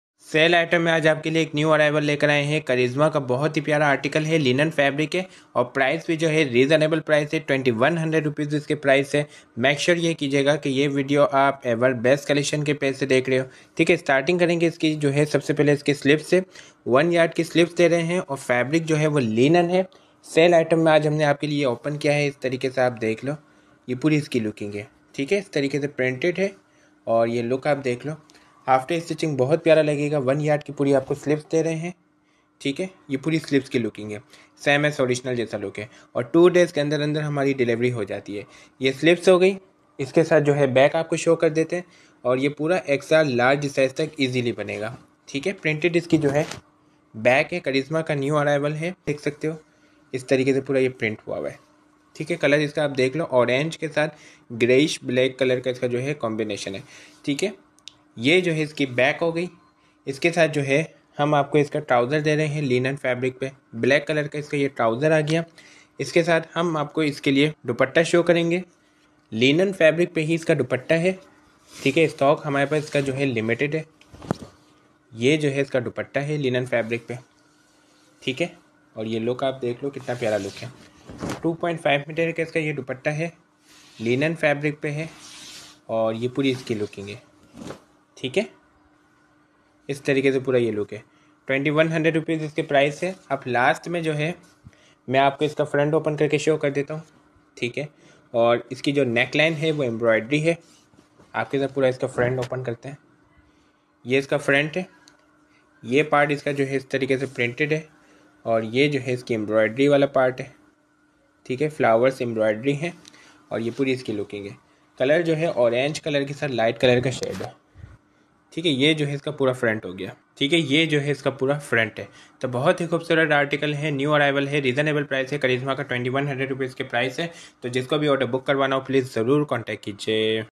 Sale Item (Charizma Linan)😍🔥🔥New Arrival 🥰💥💥Beautiful Colour 💐 MashaallahPrice:2100/-
Sale Item.
Charizma Linan😍🔥🔥
New Arrival 🥰💥💥
Beautiful Colour 💐 Mashaallah
Price:2100/- Fixed With 250/- Delivery Charges Apply Fixed rate
Fabric Linan
Dupatta Linan
Linan Trouser
Front lenght 45+
Front width 39+
2.5 meter Trouser